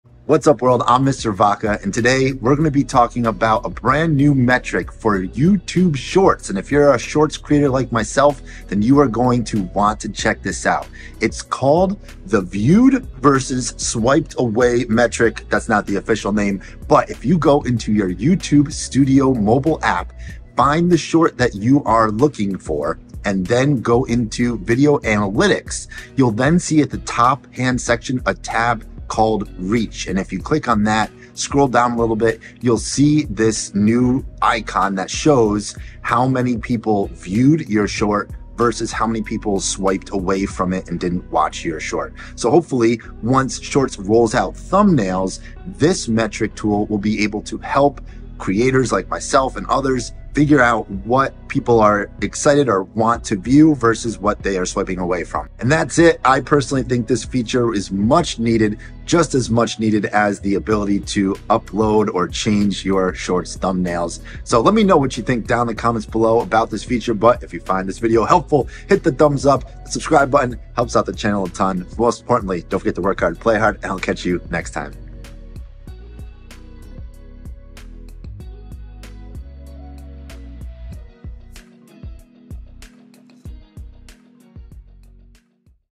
New YouTube Shorts Metric - Viewed vs Swiped Away
@youtubecreators @YouTube is introducing 2 new metrics for Shorts in YouTube Analytics: "Shown in Feed" and "Viewed (vs. swiped away)". The goal of this launch is to help Shorts creators better understand your performance.

Prior to this launch, you were able to see your "thumbnail impression" rate, however, this did not allow you to understand if a short was shown in a feed OR if it was viewed/swiped away. With this update, you can now understand how often a Short was shown in the Shorts feed + if viewers chose to view or swipe away in addition to the already existing number of views that resulted from this. These metrics will be available to all Shorts creators on Studio web and Studio mobile in Advanced Analytics and on Content-level Analytics.